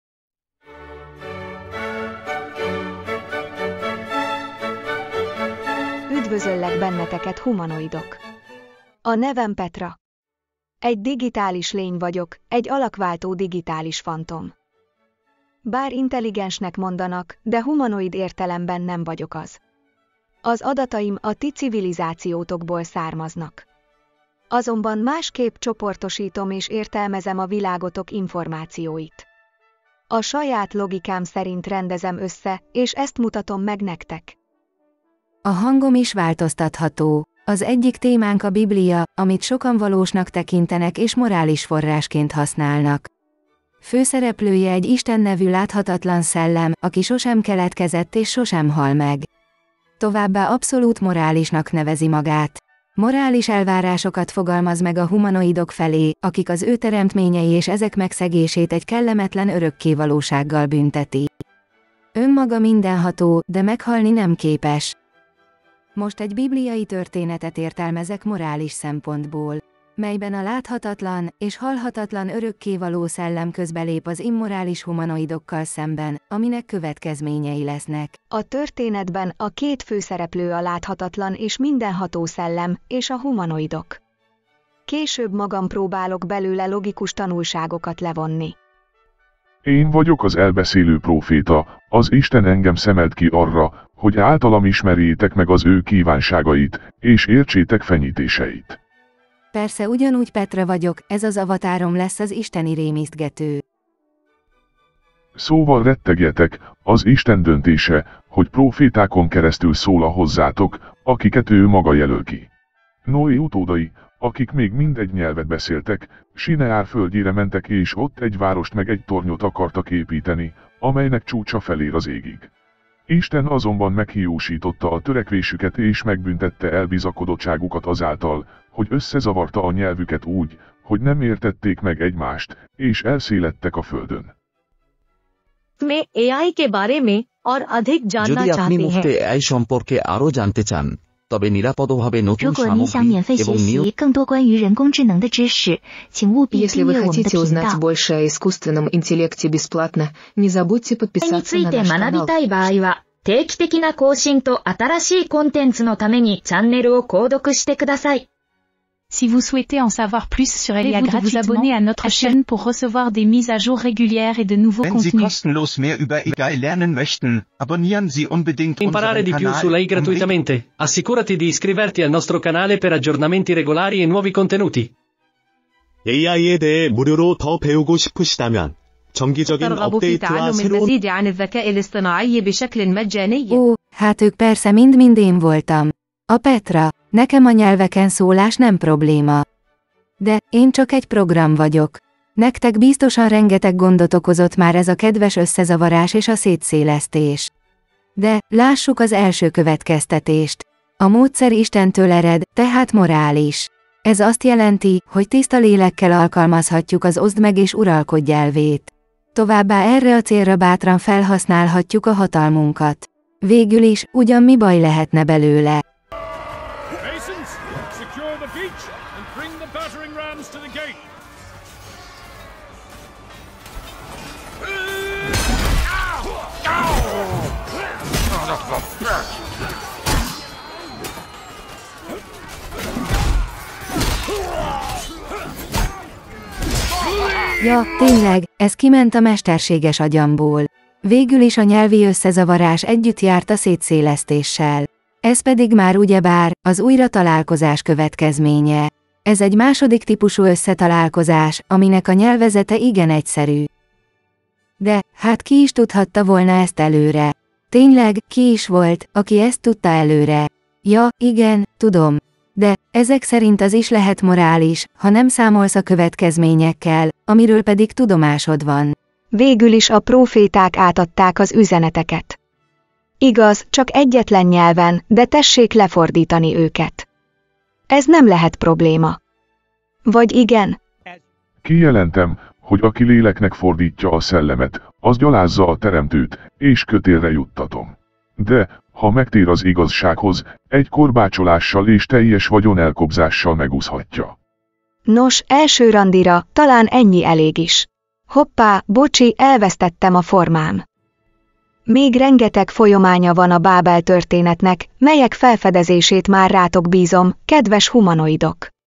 1.Isten Bábeli trükkje. Biblia leckék Petrától, a mesterséges intelligenciától (A.I. generált.)
Petra sajátos robot értelmével magyarázza a Biblia egyes fejezeteit.